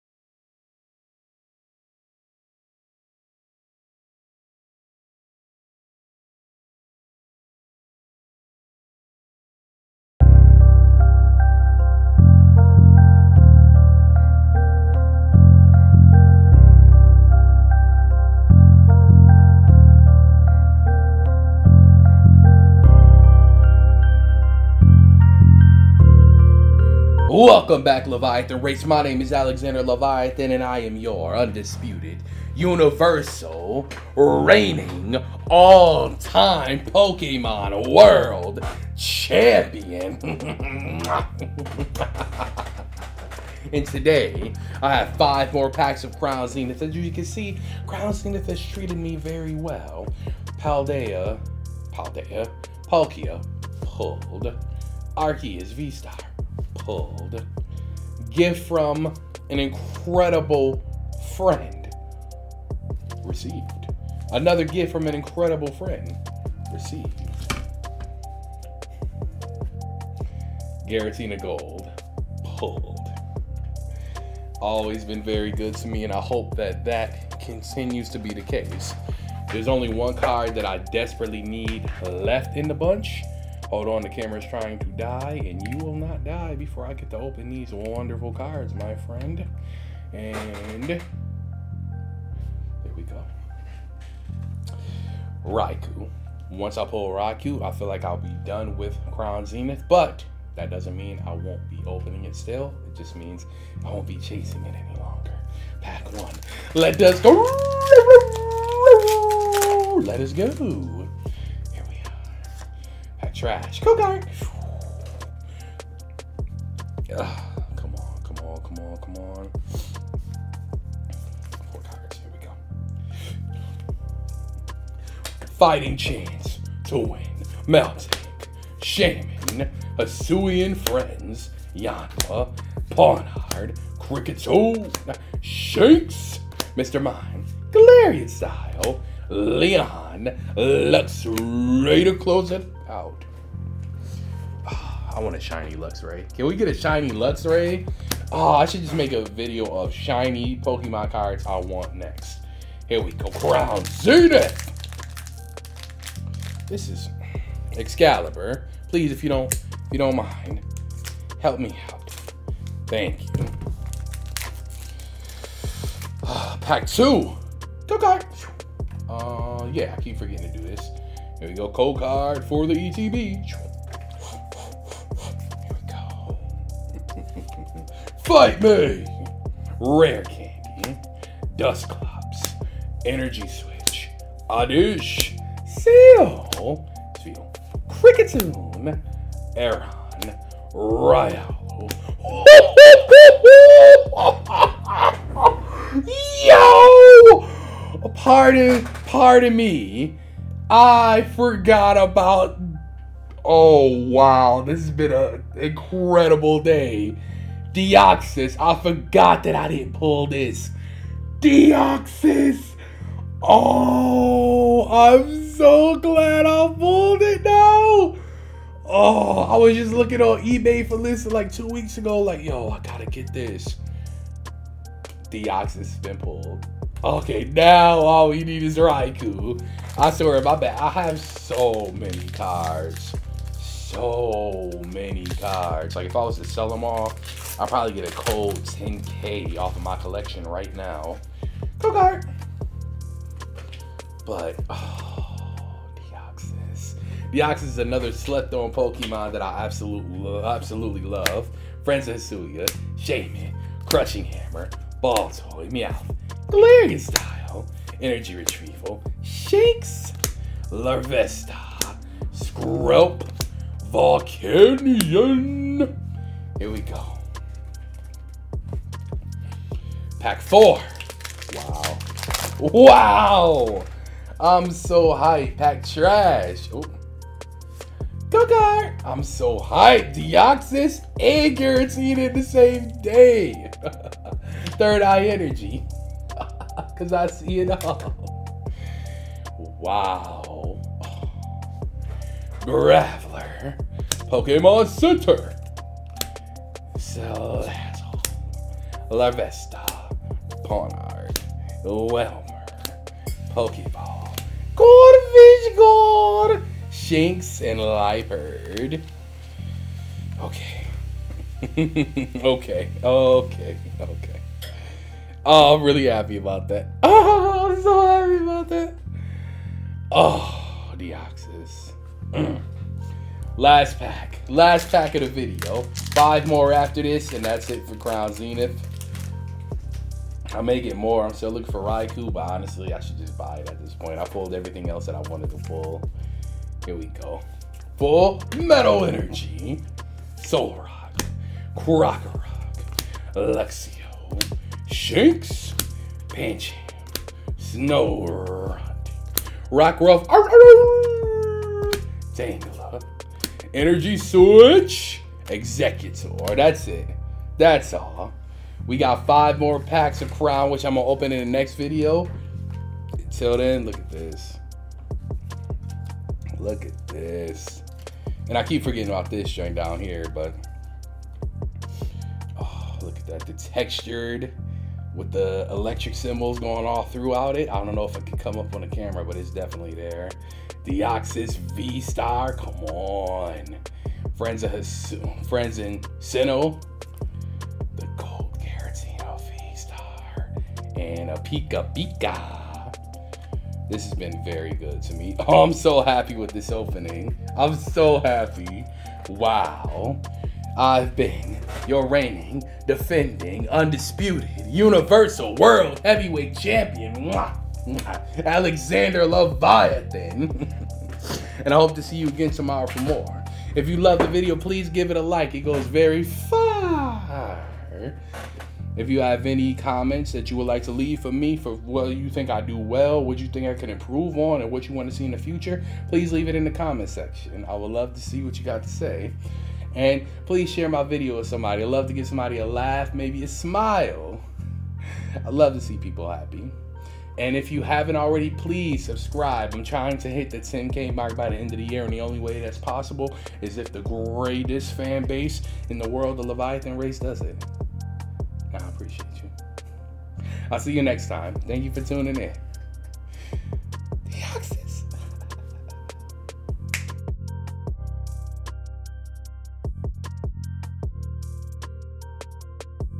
Crown Zenith ETB Opening! Part 46
Pokémon Pack Opening! What Will I Pull? Watch The Whole Video & Find Out.

FOLLOW @BrilliantToAll on Twitter

FOLLOW Lordalexander,Leviathan on Instagram

CLICK LIKE ⁣
CLICK SAVE ⁣
CLICK SHARE